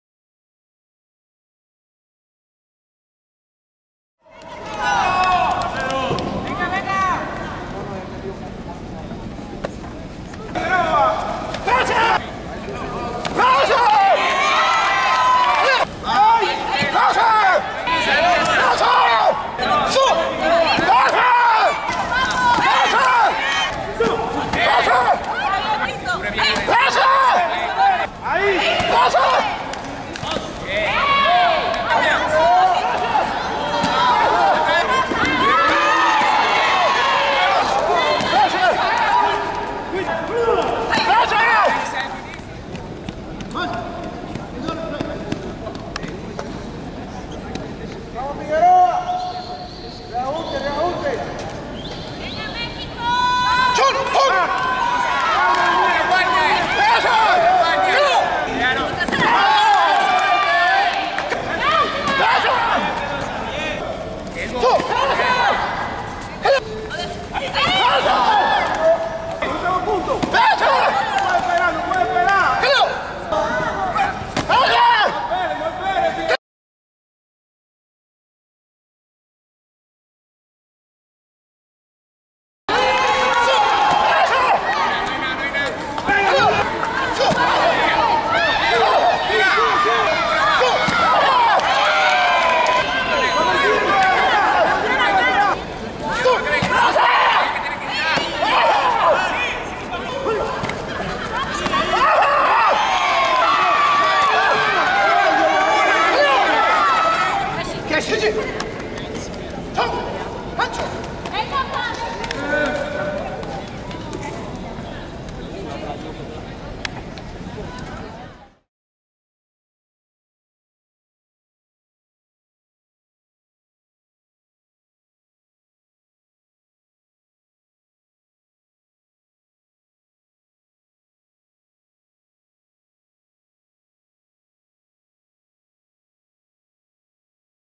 💥 Highlights de Francisco Pedrosa 🇲🇽 vs 🇩🇴 K44 Parataekwondo Male +80 kg.
Algunos de los mejores momentos del competidor mexicano en su pelea contra el dominicano Julio Armando Figuereo en la categoría +80 kg. K44 en el clasificatorio a JuegosParapanamericanos Santiago 2023 en Río de Janeiro.

Bitcoin: bc1qpvennn3ljsq23smwc2tywj6t0m7rk7jf980lyu